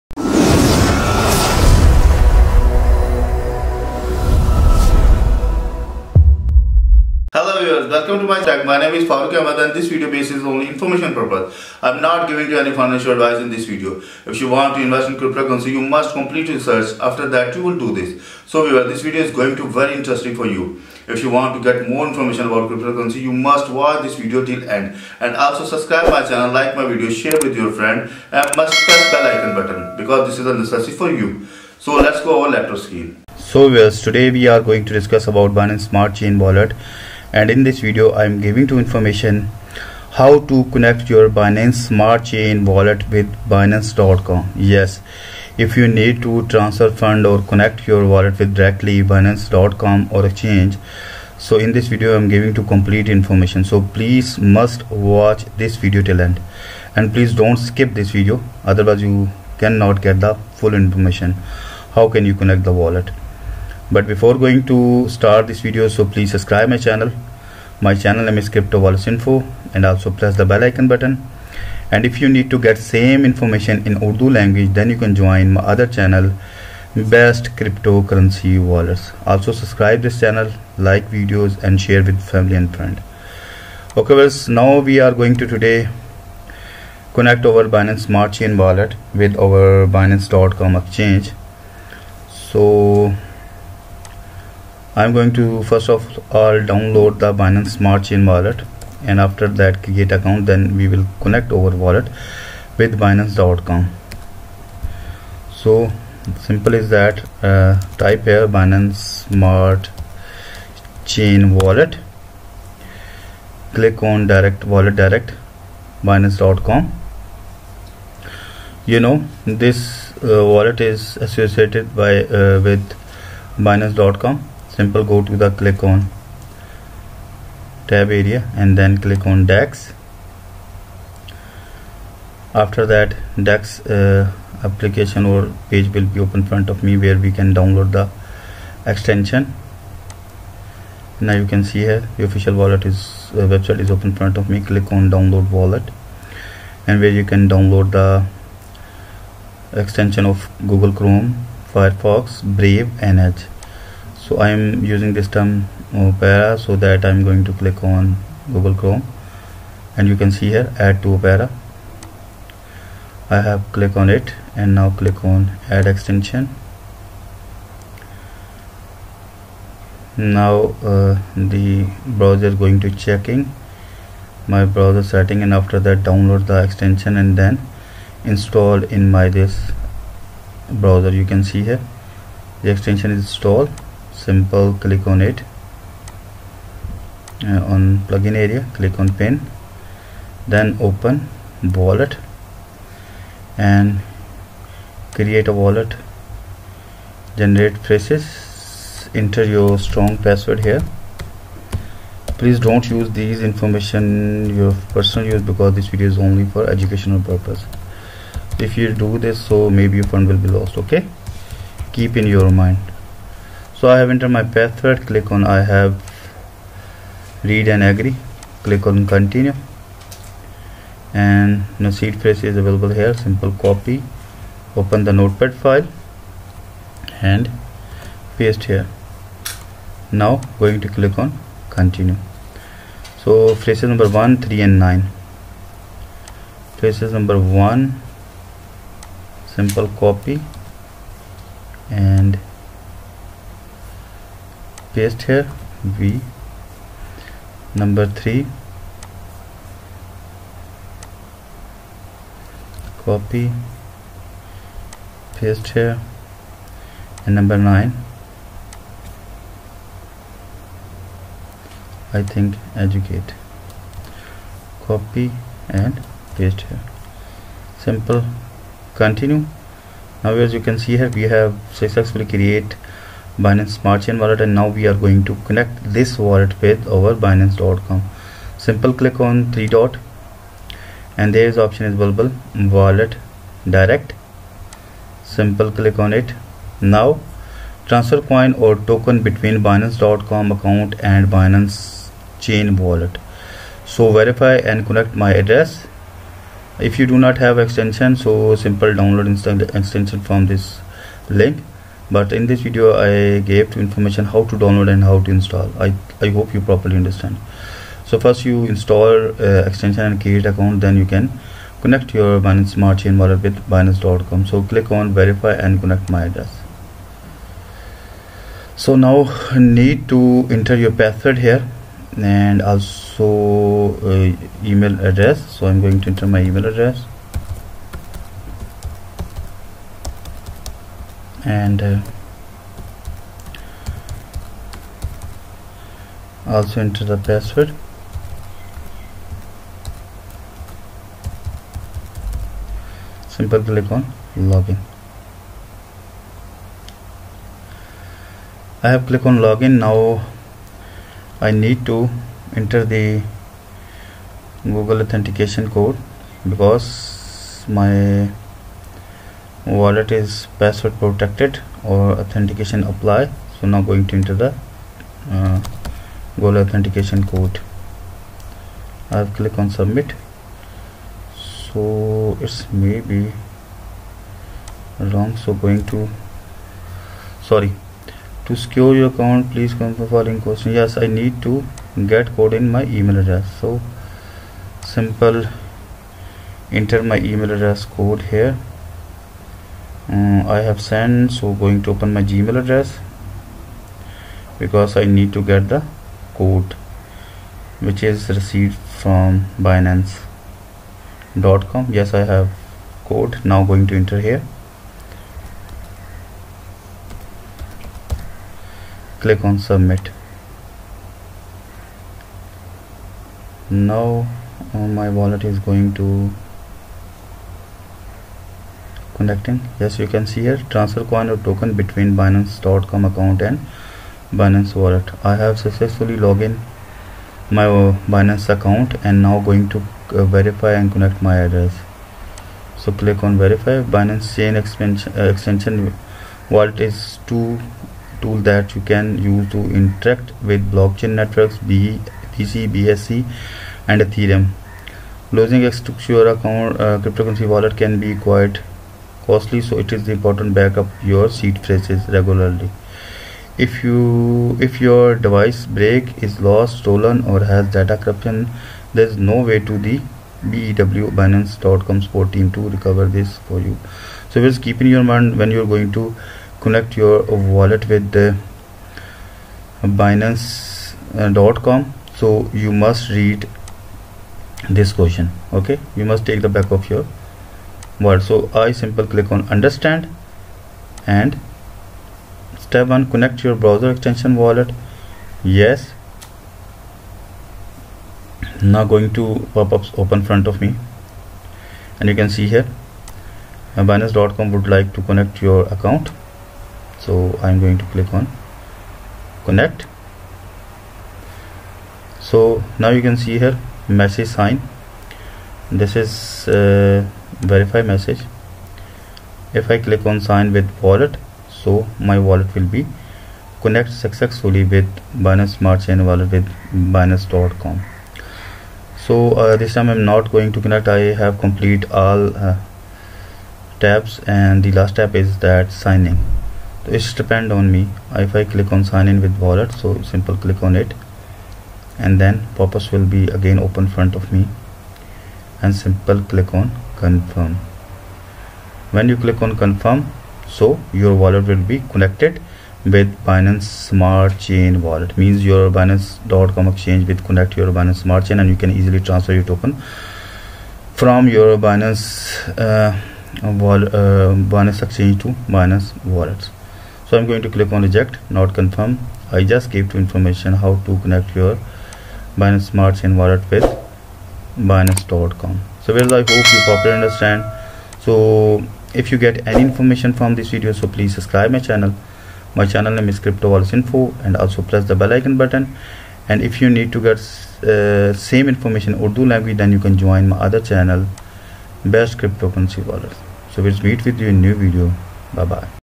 How to Connect Binance Wallet and Binance Extension Wallet (BEW) via Wallet Direct
How to Connect Binance Wallet and Binance Extension Wallet (BEW) via Wallet Direct. If you already have some BNB in your Binance spot wallet, you can connect your account with your Binance Chain Wallet using the [Wallet Direct] feature. 1. Click the icon on the top right of the wallet's homepage, and then click [Connect to Binance.com account.